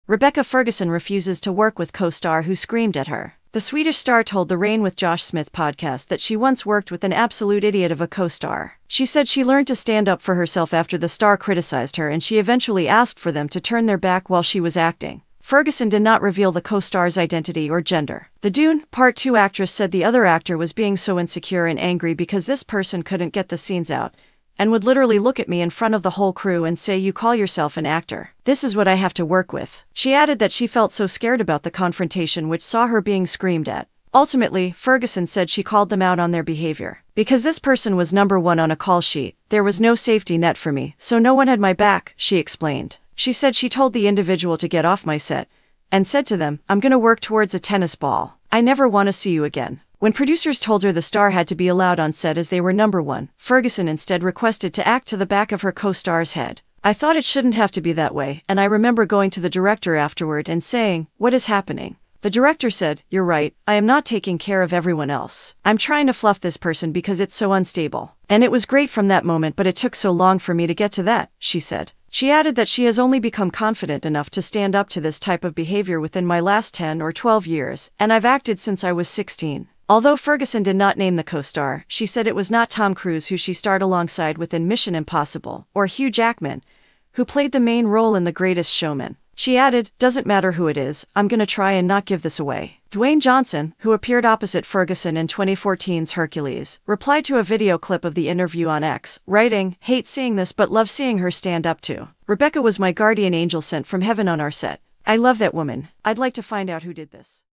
"Rebecca Ferguson Shuns Co-star's Screaming"
The Swedish star told the Reign with Josh Smith podcast that she once worked with an "absolute idiot of a co-star".
She said she learned to stand up for herself after the star criticised her and she eventually asked for them to turn their back while she was acting.
Ferguson did not reveal the co-star's identity or gender.
The Dune: Part Two actress said the other actor was "being so insecure and angry because this person couldn't get the scenes out" and "would literally look at me in front of the whole crew and say 'You call yourself an actor? This is what I have to work with?'"
She added that she felt "so scared" about the confrontation which saw her being "screamed at". Ultimately, Ferguson said she called them out on their behaviour.
"Because this person was number one on a call sheet, there was no safety net for me, so no one had my back," she explained.
She said she told the individual to "get off my set", and said to them: "I'm gonna work towards a tennis ball. I never want to see you again."
When producers told her the star had to be allowed on set as they were "number one", Ferguson instead requested to act to the back of her co-star's head.
"I thought it shouldn't have to be that way, and I remember going to the director afterward and saying, 'What is happening?'
"The director said, 'You're right, I am not taking care of everyone else. I'm trying to fluff this person because it's so unstable.'
"And it was great from that moment but it took so long for me to get to that," she said.
She added that she has only become confident enough to stand up to this type of behaviour "within my last 10 or 12 years, and I've acted since I was 16".
Although Ferguson did not name the co-star, she said it was not Tom Cruise who she starred alongside with in Mission Impossible, or Hugh Jackman, who played the main role in The Greatest Showman.
She added: "Doesn't matter who it is, I'm gonna try and not give this away."
Dwayne Johnson, who appeared opposite Ferguson in 2014's Hercules, replied to a video clip of the interview on X, writing: "Hate seeing this but love seeing her stand up to [it].
1.
2.
3.
4.
5.
6.
7.
8.
9.
10.
11.
12.
13.
14.
15.
16.
17.
18.
19.
20.
21.
22.
23.
24.
25.